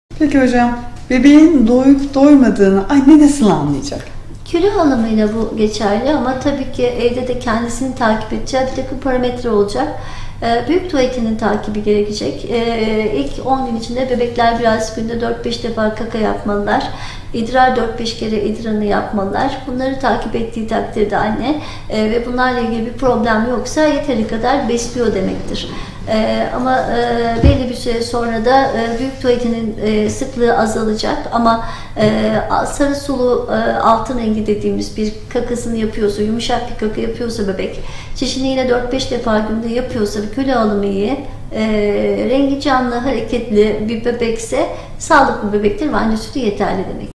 Çocuk Hastalıkları ve Sağlığı Uzmanı Dr. Ersin Sarı '' Bebeklerde Beslenme '' anlatıyor.
Avrasya Hastanesi Çocuk Hastalıkları ve Sağlığı Uzmanı Dr. Ersin Sarı  '' Bebeklerde Beslenme '' hakkında detayı bilgiler veriyor